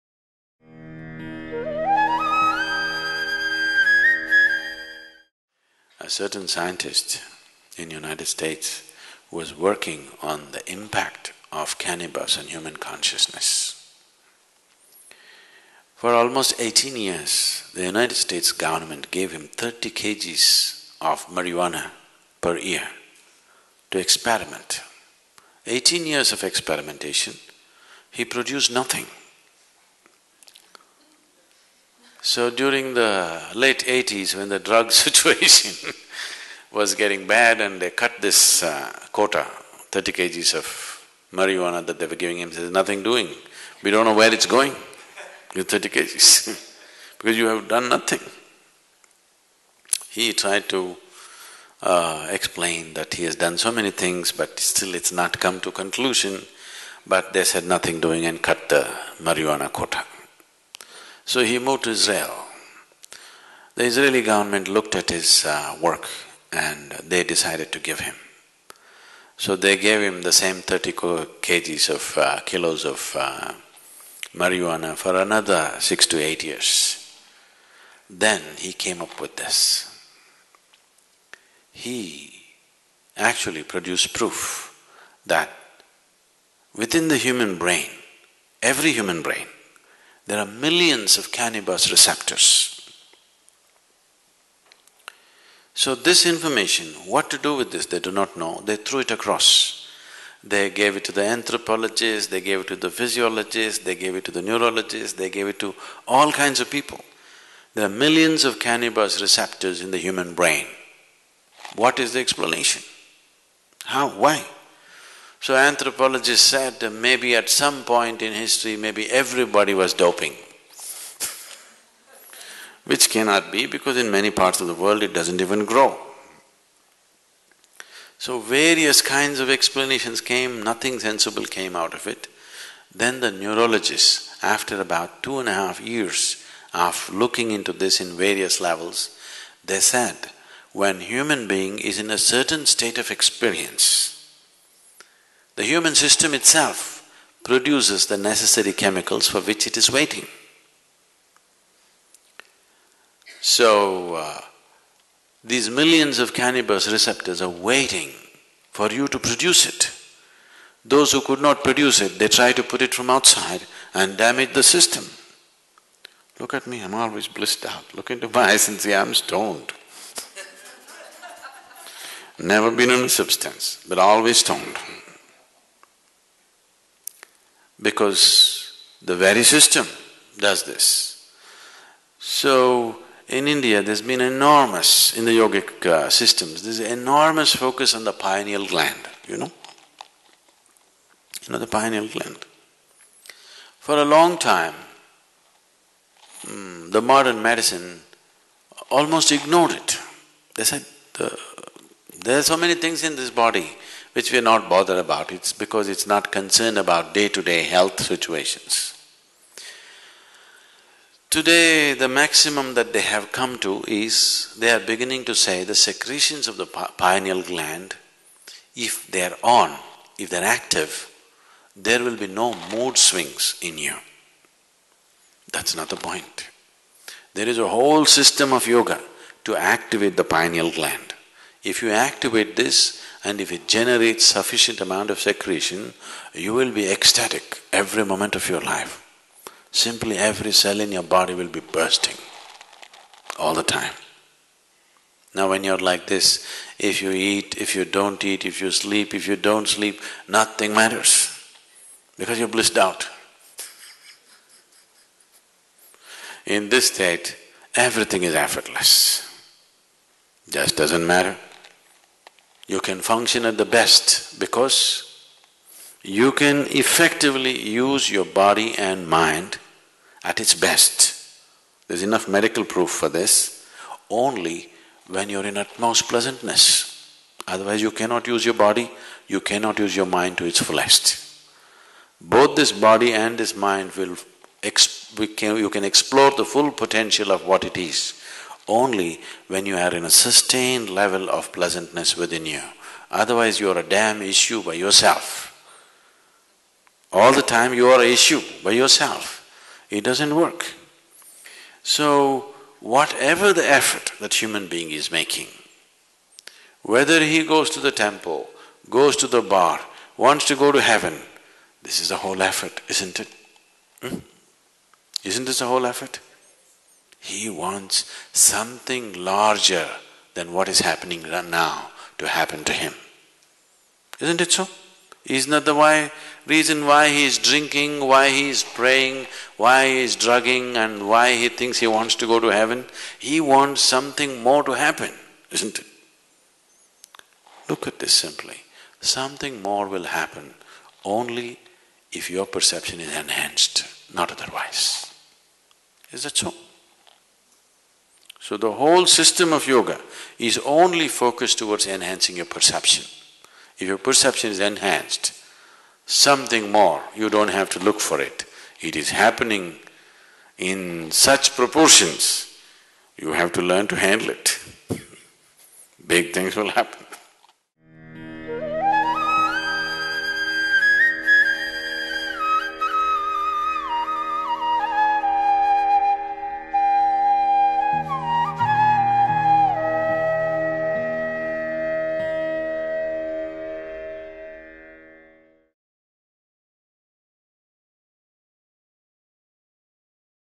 Sadhguru on Getting Stoned, Without Drugs
Detailing extensive experiments by an Israeli scientist who discovered thousands of "cannabis receptors" in the brain, Sadhguru explains that the human system itself is capable of producing such chemicals on its own. Activating the pineal gland, he says, is a fundamental part of yogic practices, through which one can be "stoned" by his own nature -- without side effects.

Sadhguru Talks @ Sathsang